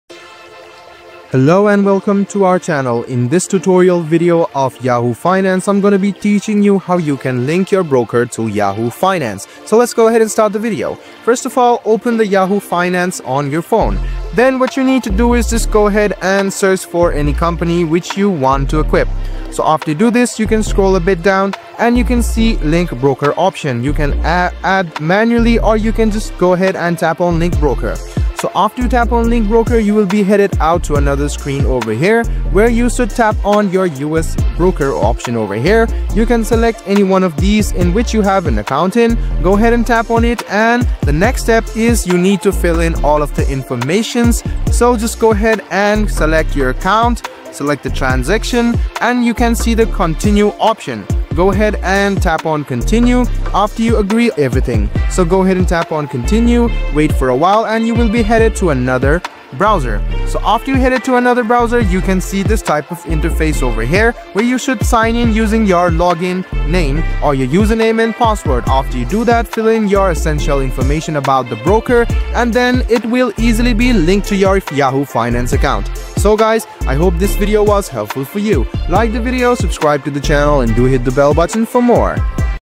How To Link A Broker To Yahoo Finance
In this tutorial, I will show you how to link your brokerage account to Yahoo Finance. This will allow you to track your investments and sync your portfolio in real-time.

I hope this guide helps you connect your broker to Yahoo Finance easily.